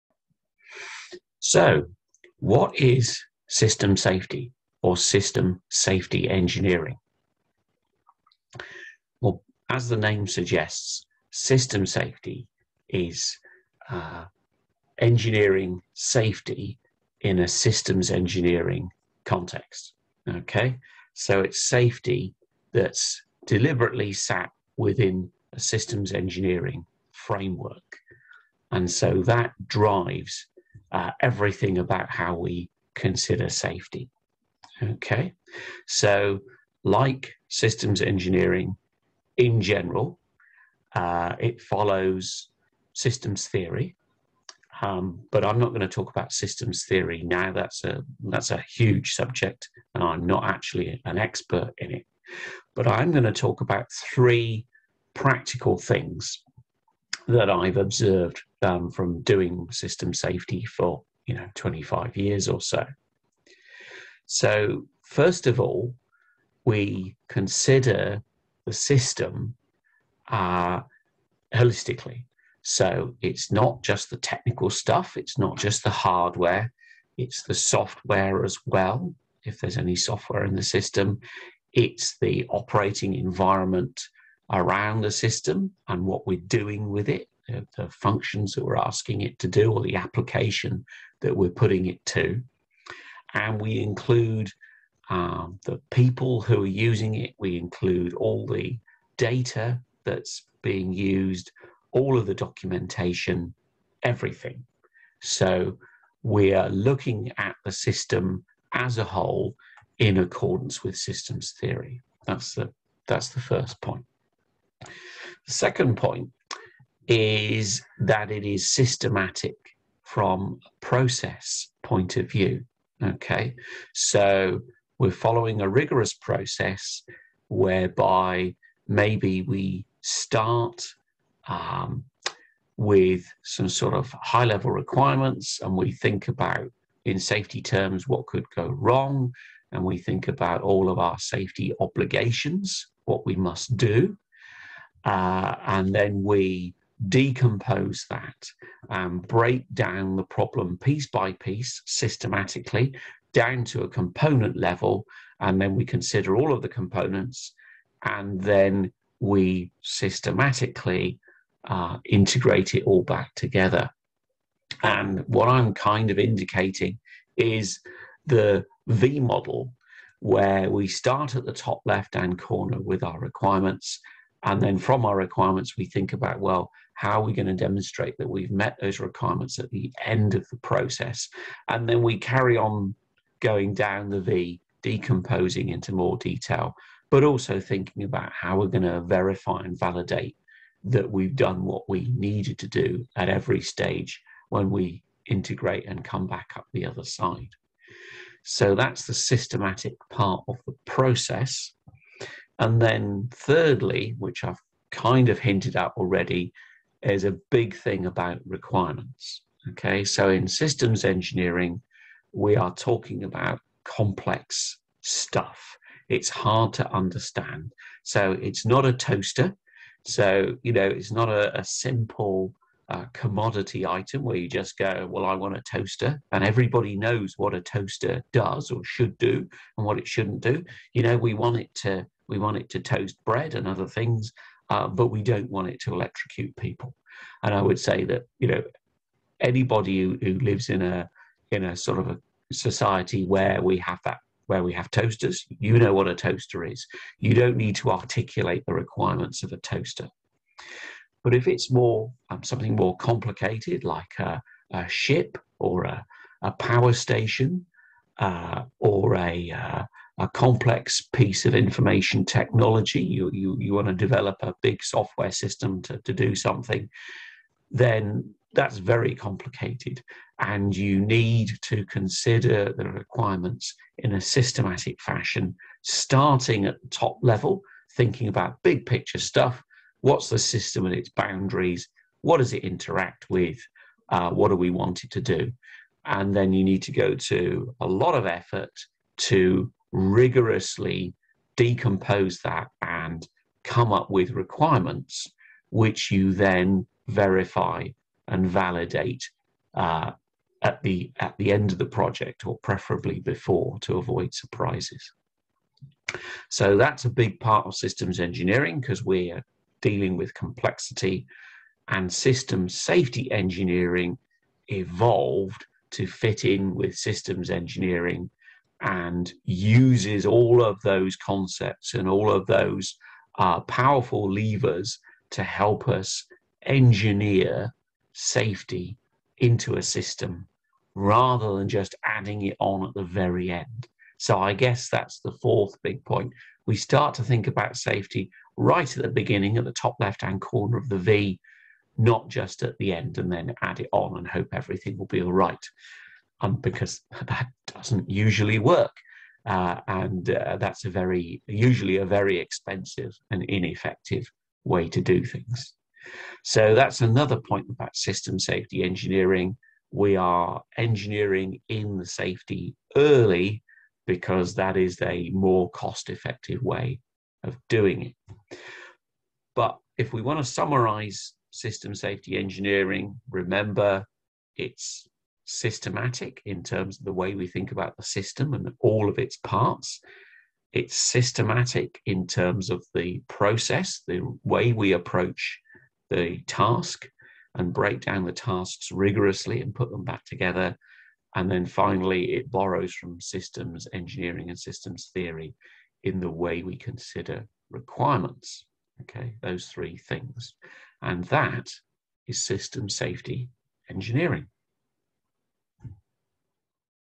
What is System Safety Engineering?
System Safety Engineering does five things:
1.  Deals with the whole system, including software, data, people, and environment;
2.  Uses a systematic (rigorous) process;
3.  Concentrates on requirements (to cope with complexity);
4.  Considers safety early in the system life cycle; and
5.  Handles complexity cost-effectively and efficiently.